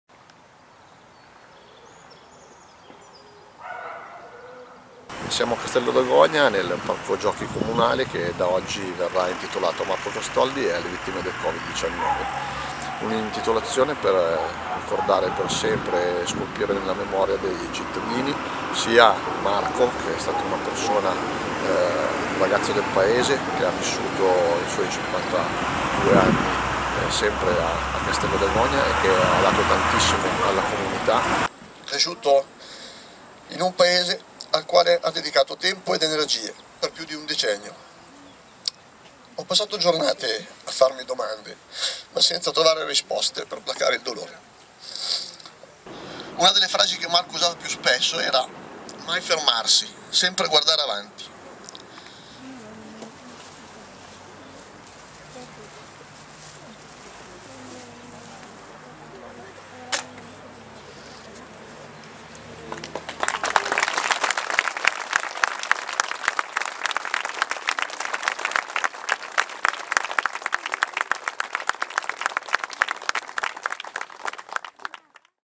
Castello d'Agogna ricorda Marco Castoldi e le vittime del Covid
Il parco giochi di via Milano è stato intitolato a Marco Castoldi (consigliere comunale 52enne stroncato dal virus) e alla vittime del Covid-19. Nel video, le parole del sindaco William Grivel (video Morandi)